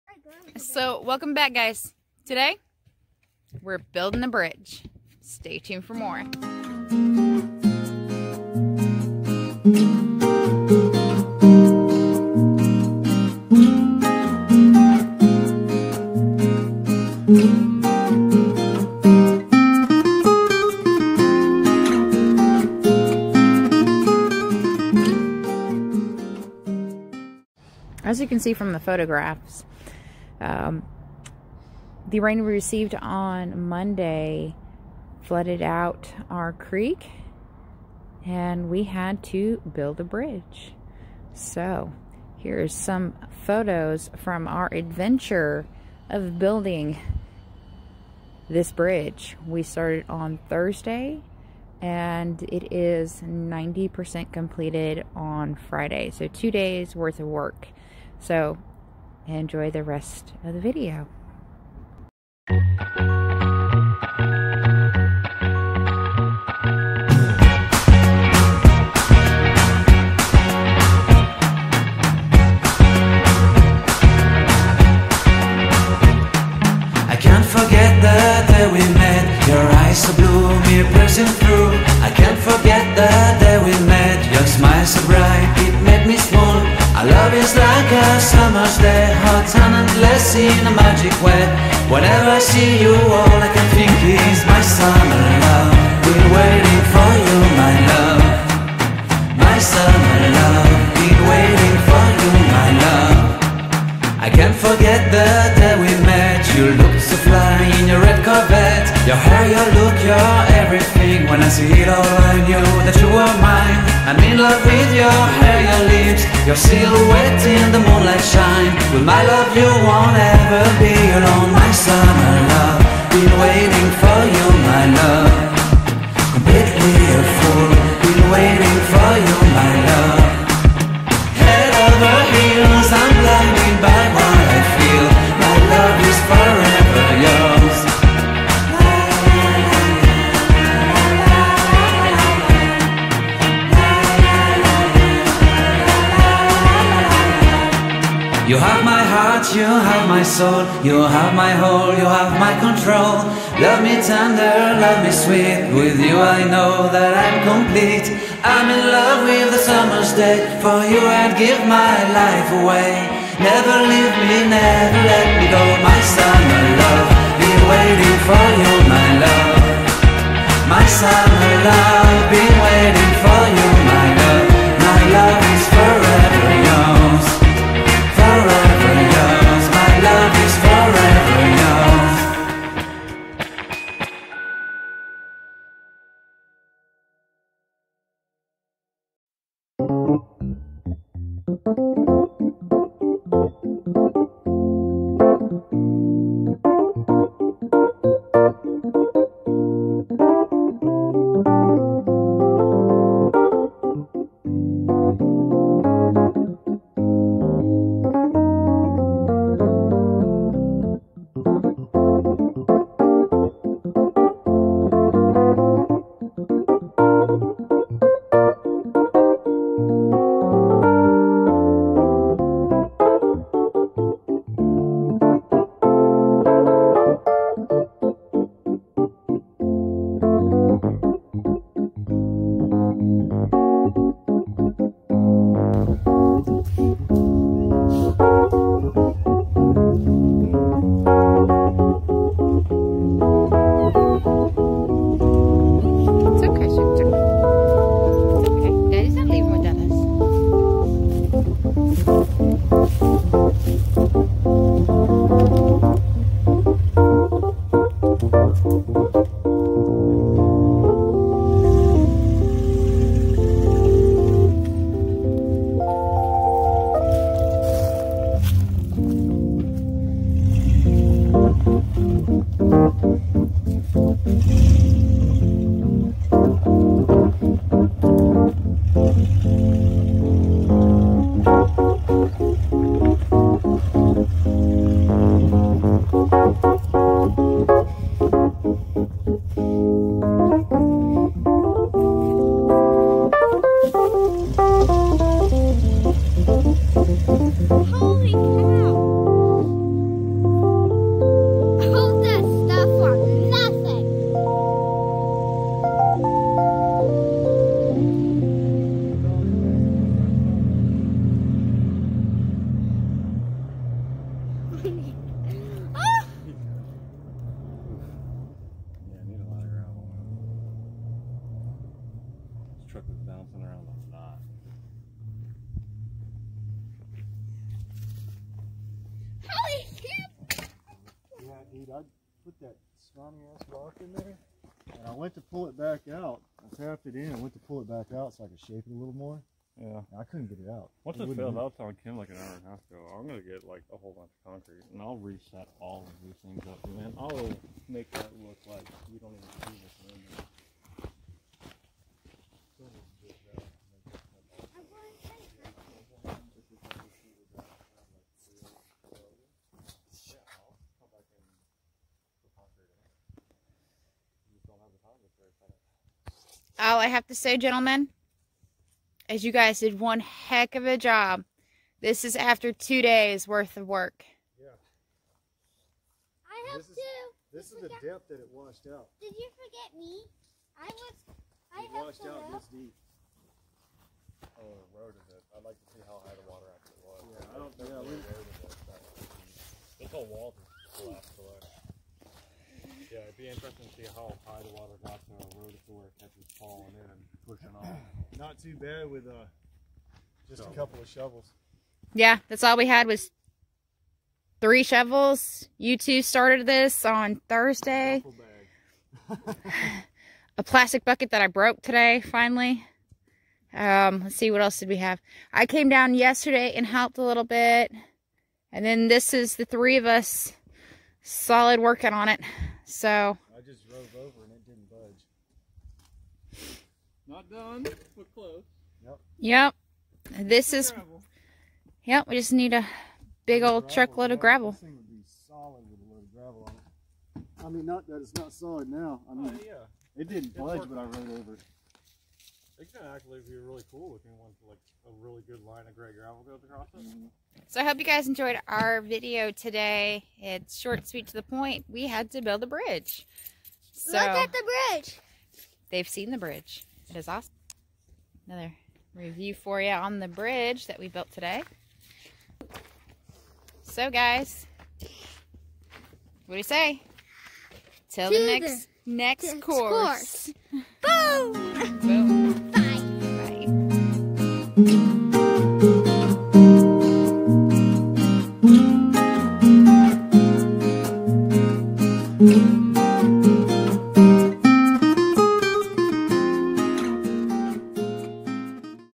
No Bridge, No Problem 8 Tons of Earth Later (No Electric Needed)(Off Grid Kentucky Homestead)
After a massive rain storm our wet creek got washed out. Trailer needs to be moved onto the property and the truck needs to be moved out. We built a bridge. 8 tons of earth later we got it about 90% complete. We are ready for moving day.